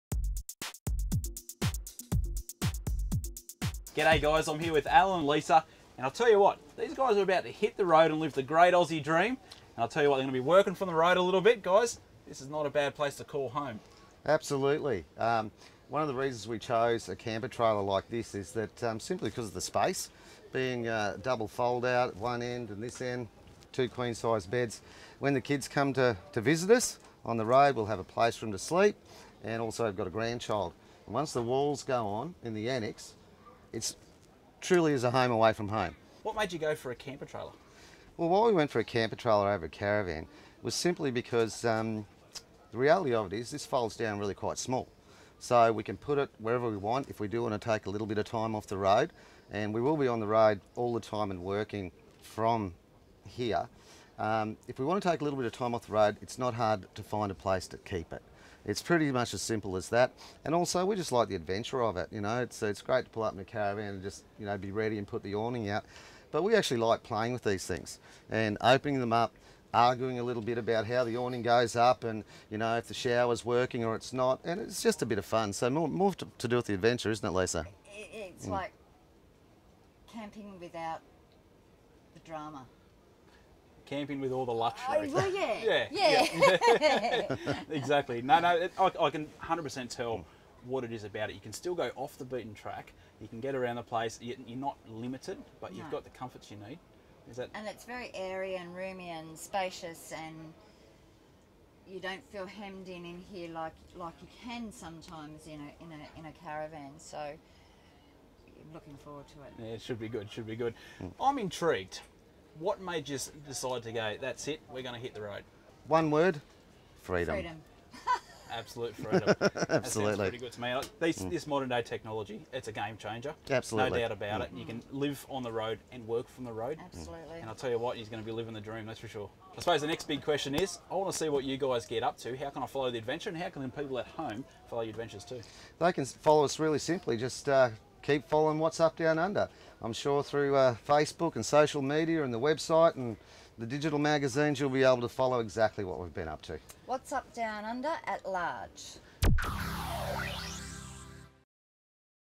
Allan and Lisa Goldby from What's Up Downunder choose MDC
Borgy chats with Allan and Lisa Goldby about their decision to go on the road and choosing Market Direct Campers.